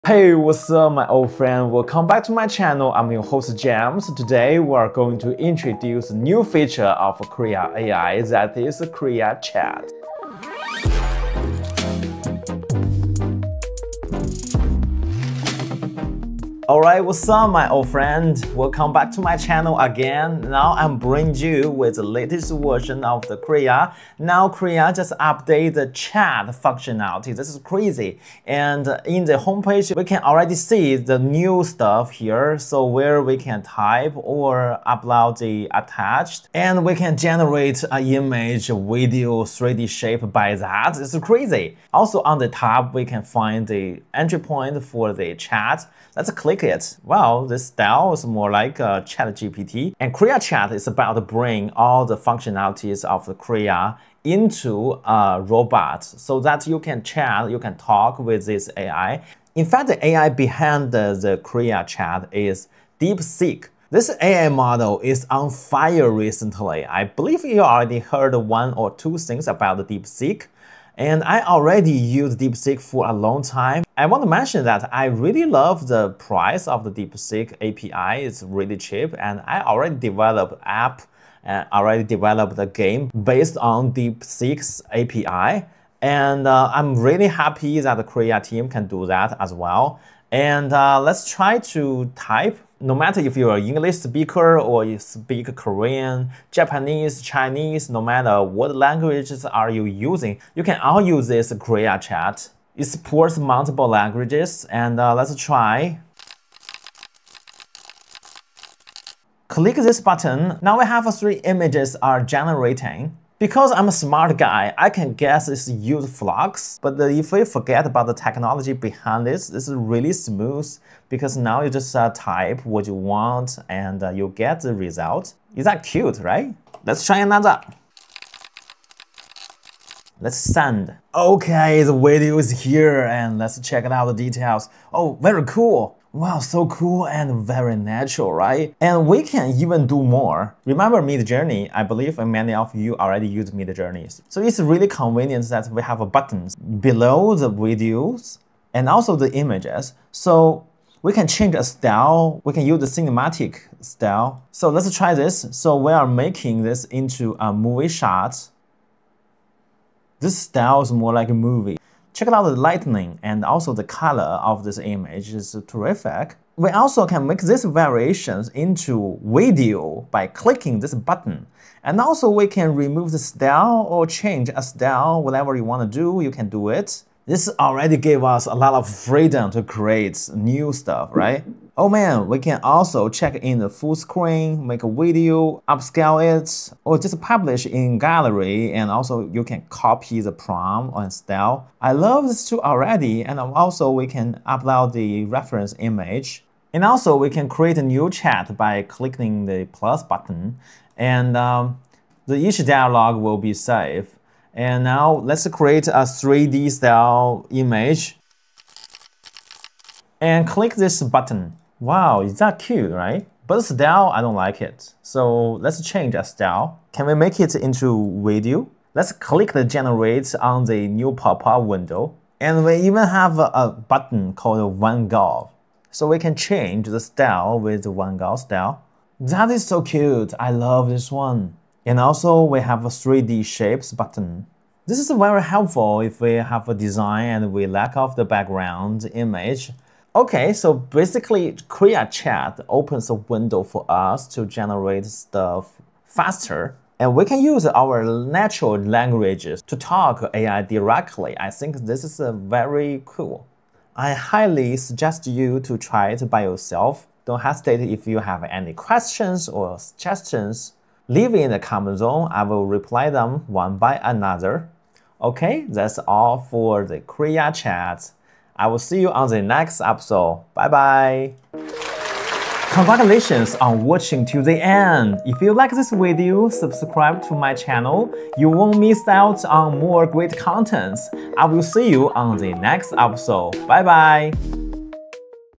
Krea AI: Krea Chat is here! - The one who beat up Midjourney
krea.ai

Krea AI is an innovative real-time image generation AI tool, designed to transform the way we create and visualize digital content. Utilizing advanced artificial intelligence algorithms, Krea AI specializes in generating high-quality images instantly based on textual descriptions. Whether you need illustrations, graphic designs, or visual concepts, Krea AI can create them in real time, catering to a diverse range of creative needs.

UX Bootcamp, updating free design & AI tutorials for everyone!